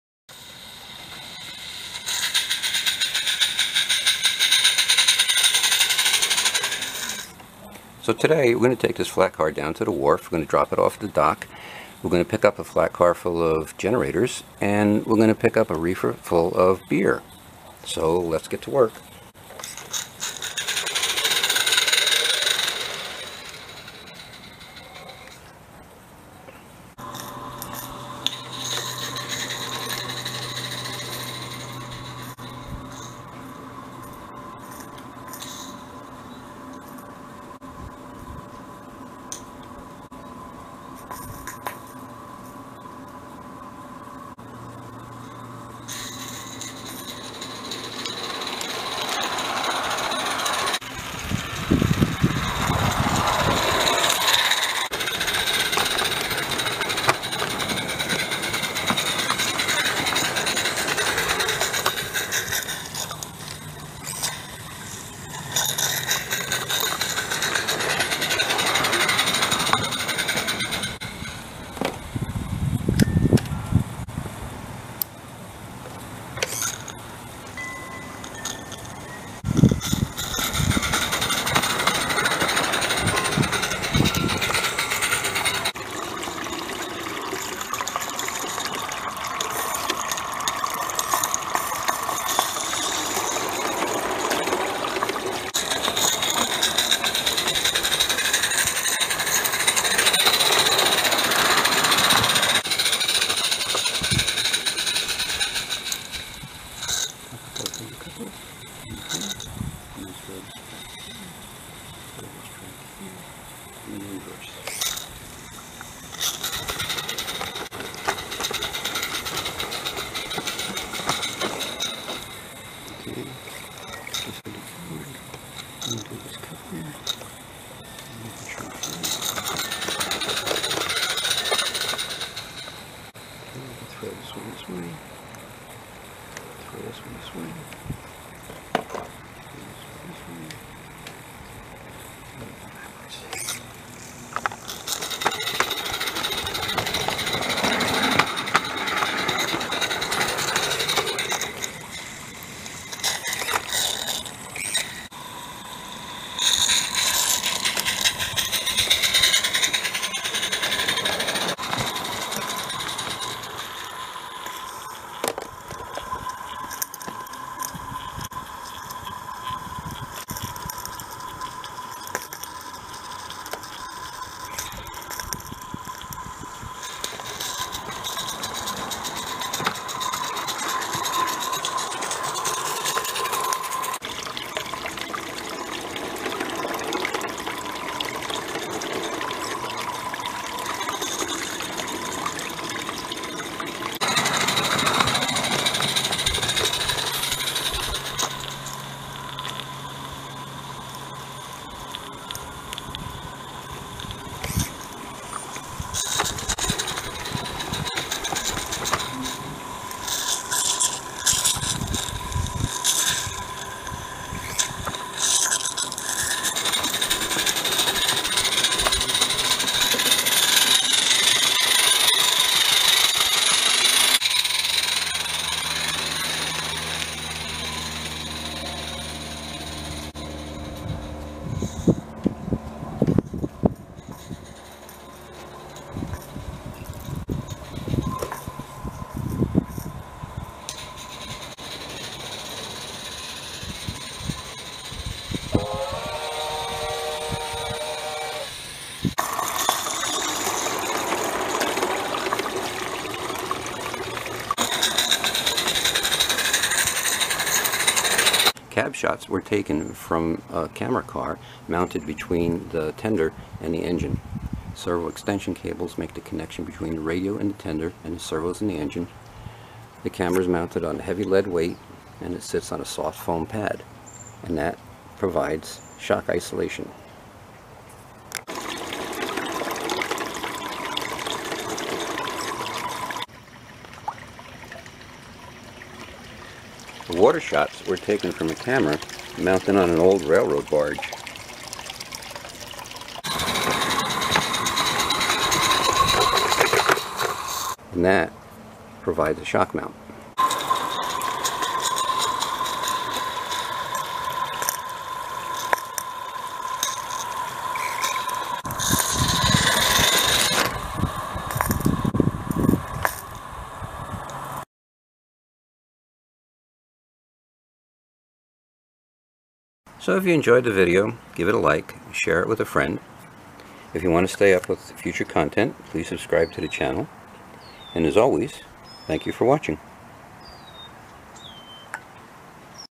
Dockside railroading, live steam Accucraft Ruby switching freight on waterfront switching maneuvers
Dockside railroading with switching maneuvers to spot freight cars on sidings. An empty flatcar needs to be delivered to the dock, but a flatcar of generators is currently there. Several switching maneuvers are required to exchange the cars so the empty could be spotted on the dock for loading, and a car with generators and a refer of beer picked up. A live steam Ruby locomotive was used as the dockside switcher.